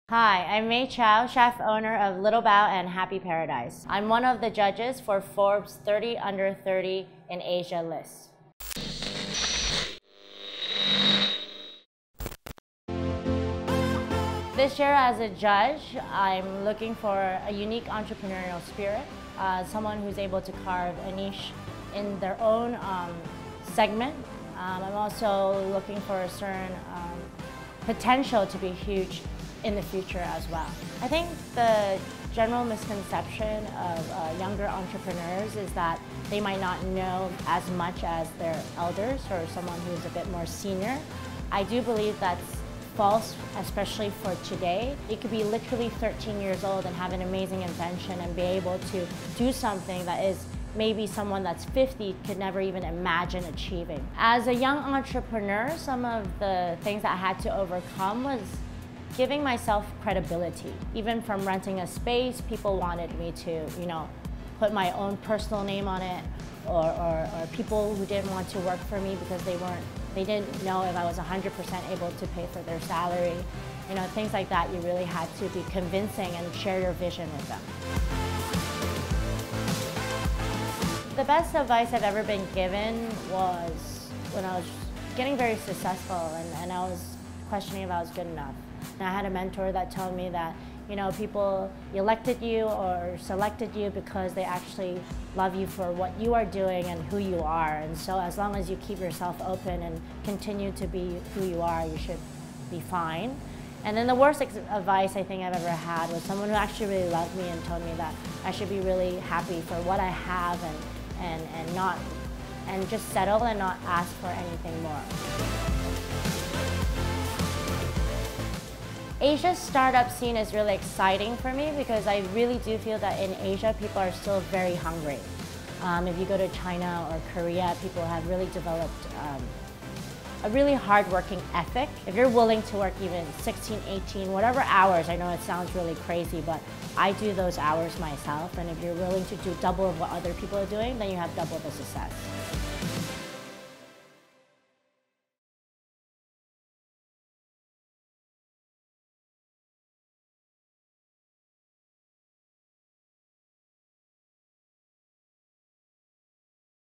May Chow’s Recipe for Success
Restaurant owner May Chow, who was voted Asia's Best Female Chef 2017, was one of the judges for this year's 30 Under 30 Asia list. She told us what she was looking for from the entries, and also about her own experience as a young entrepreneur.

Forbes Asia covers Asia's business leaders and entrepreneurs.